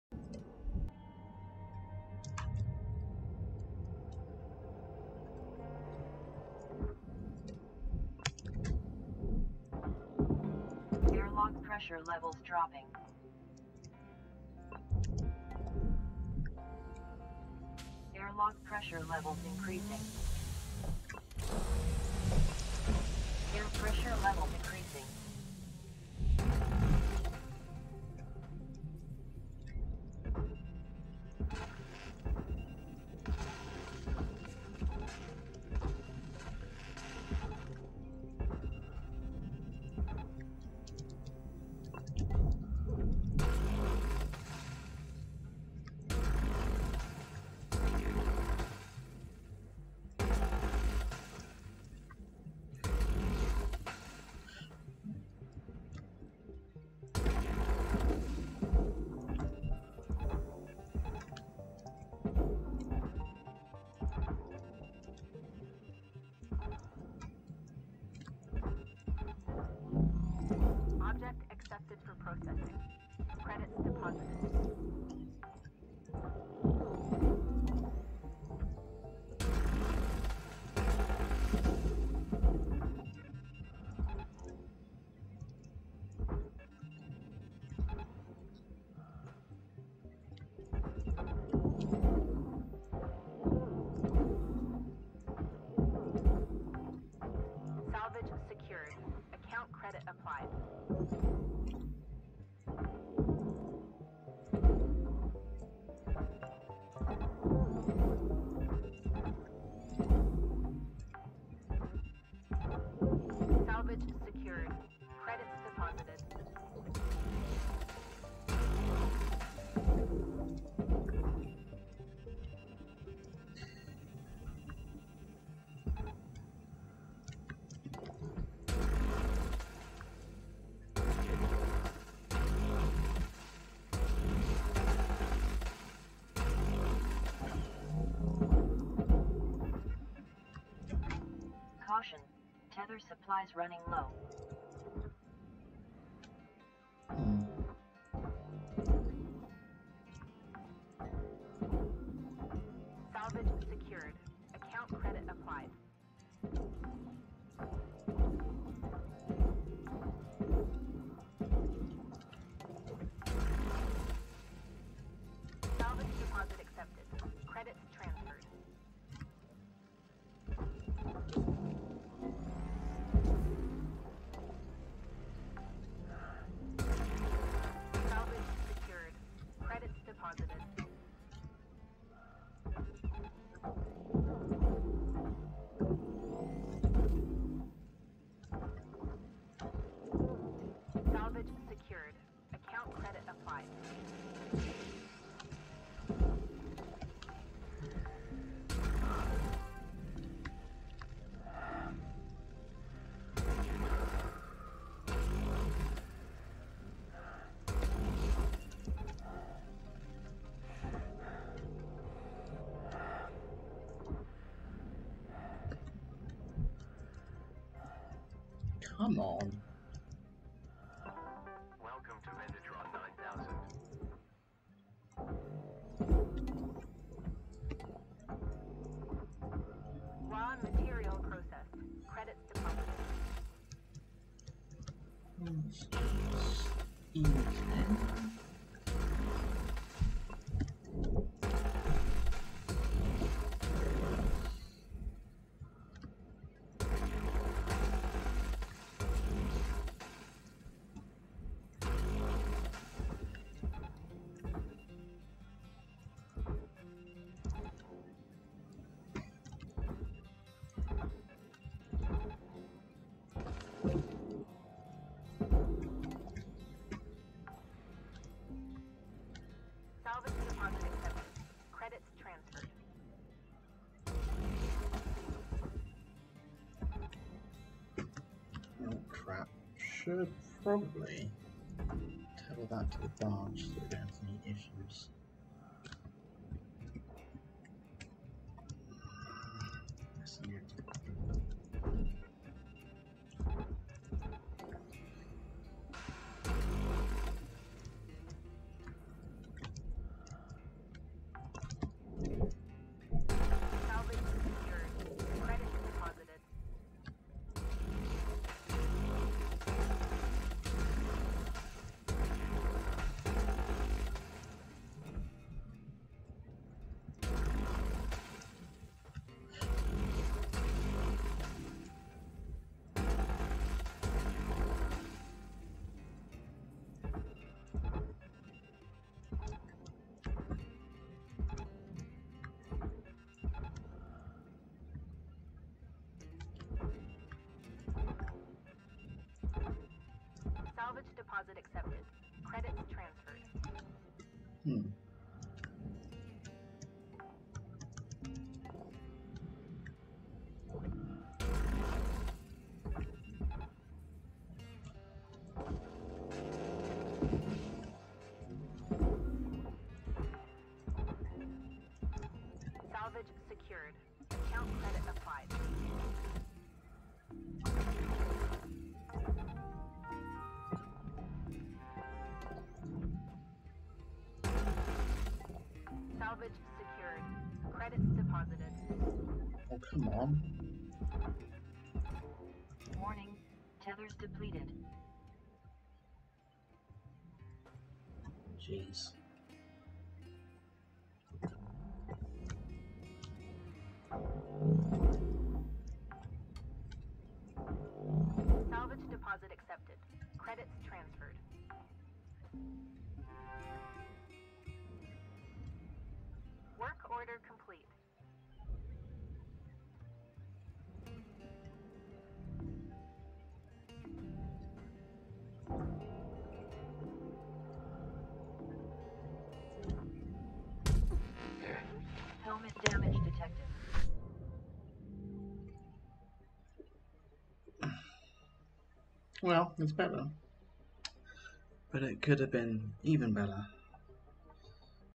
RACE week #16 No1 score (8/12)
Rapid disassembly of a Station Hopper Mackerel. Current #1 legitimate score!